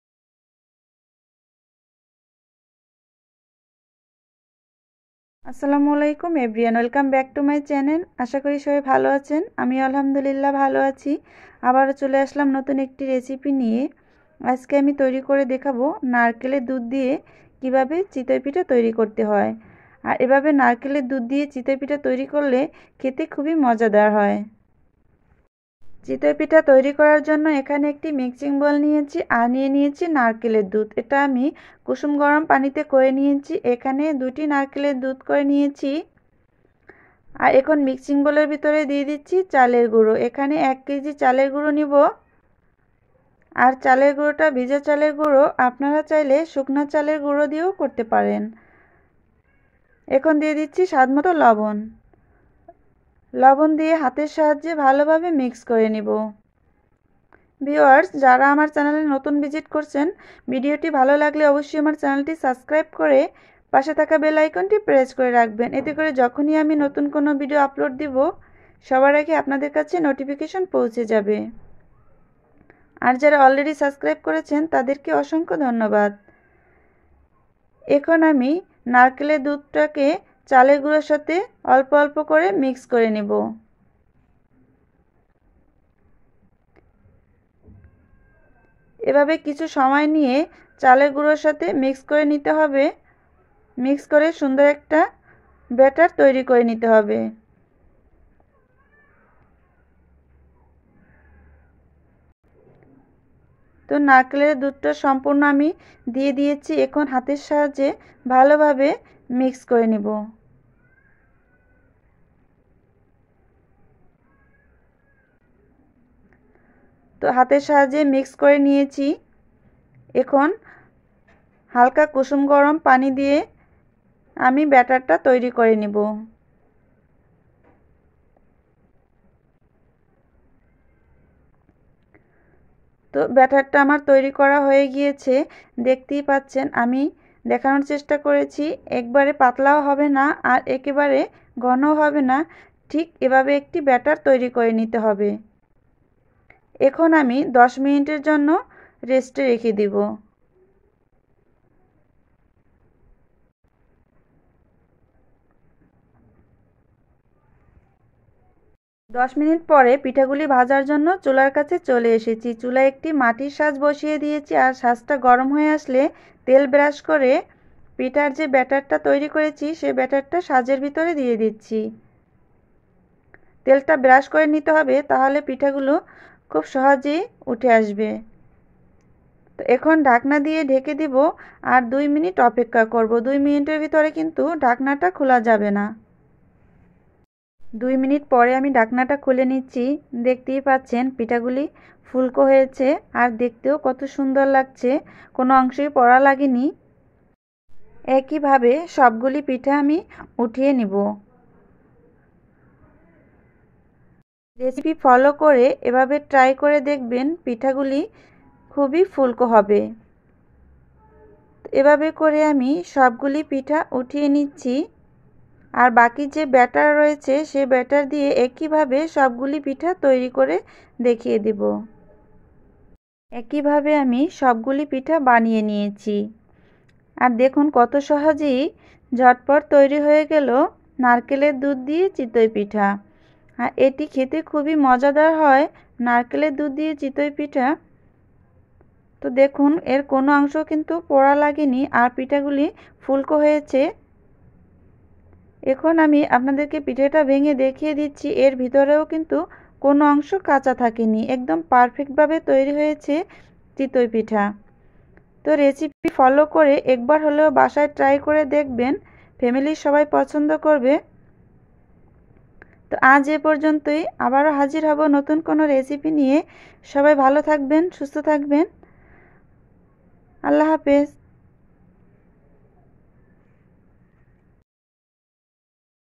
চালের গুড়ো আর নারিকেল এর দুধ দিয়ে ফুলকো চিতই পিঠা রেসিপি/Chitoi Pitha Recipe
উপকরণ:
চালের গুড়ো- ১ কেজি
২-টি নারিকেল এর দুধ
স্বাদ মতো লবণ
কুসুম গরম পানি।

* চালের গুড়ো আর নারিকেল এর দুধ আপনাদের পরিমাণ মতো দিতে পারবেন।